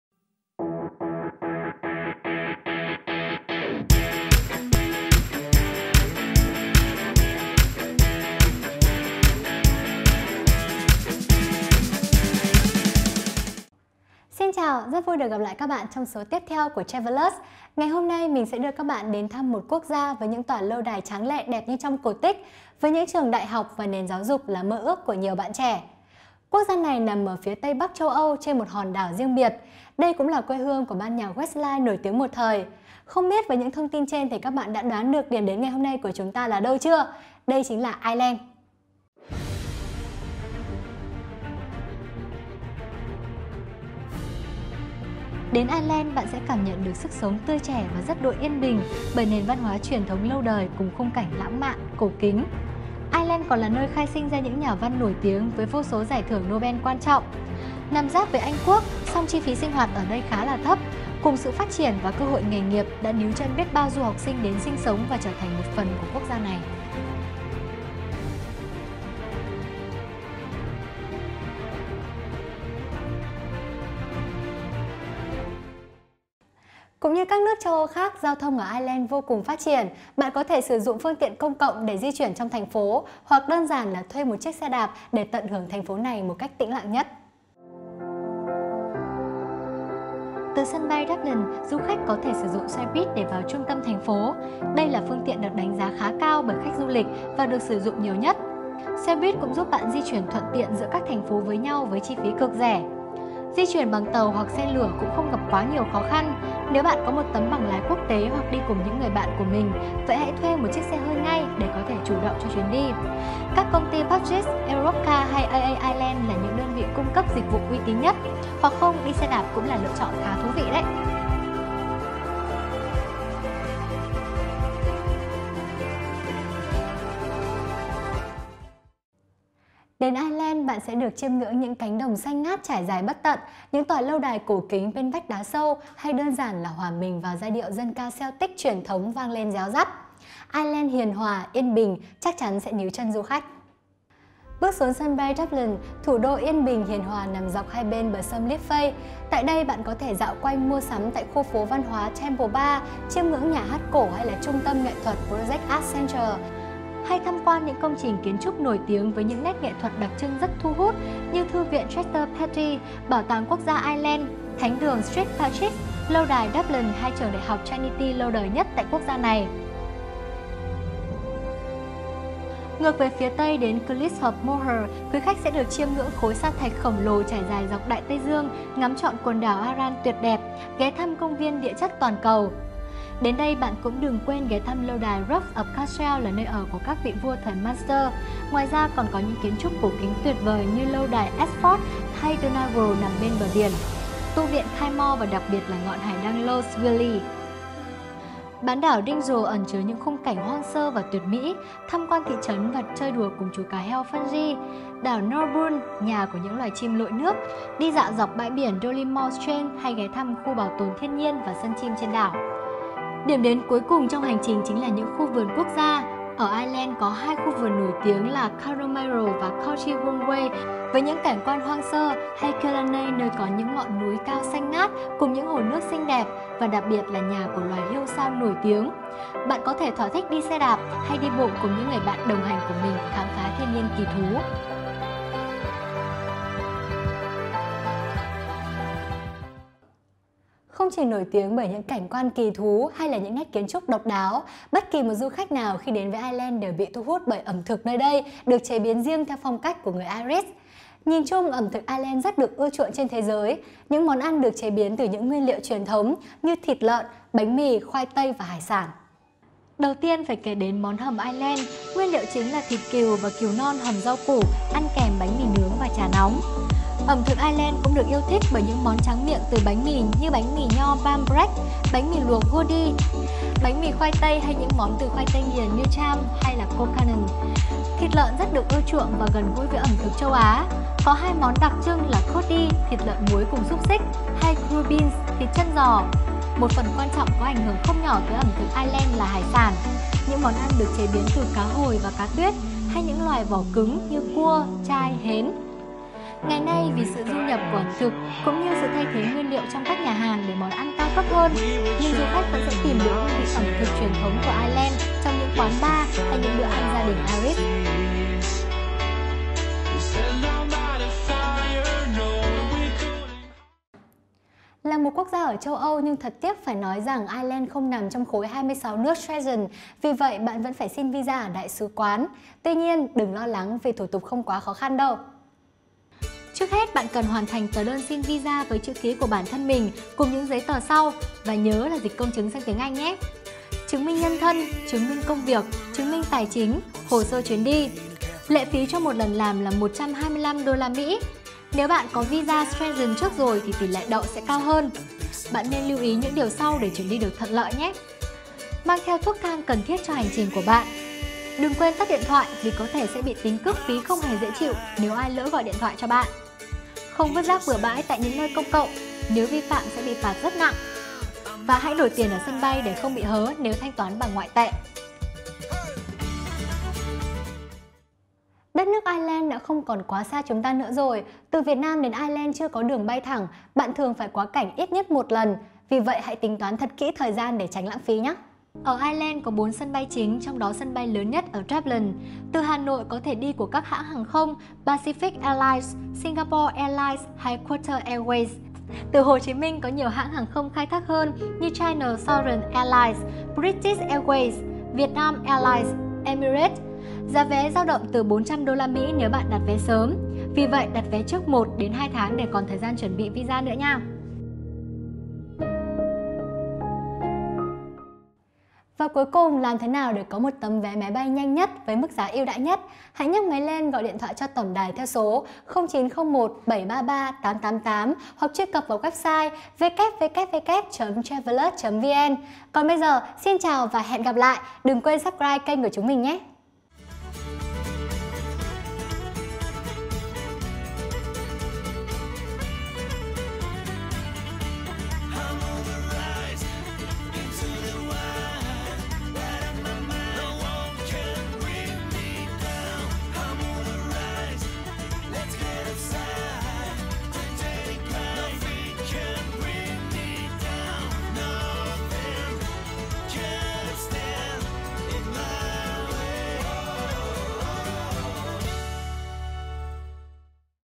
Vé máy bay đi Ireland trải nghiệm du lịch tuyệt vời cùng - Travelus
Với vé máy bay đi Ireland bạn sẽ được chiêm ngưỡng những cánh đồng xanh ngát trải dài bất tận hay các tòa nhà cổ kính trên vách đá, những giai điệu dân ca Celtic truyền thống . Ireland hiền hòa bình yên chắc chắn sẽ là điểm đến có thể níu chân du khách.